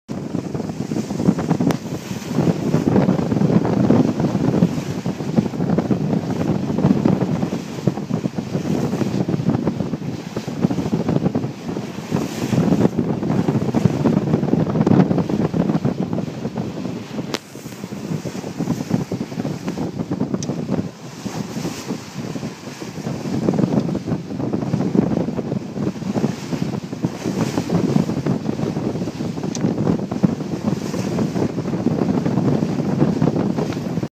Sailing on the Schooner Ondine
Sailing from Charleston to New Smyrna Beach in June 2013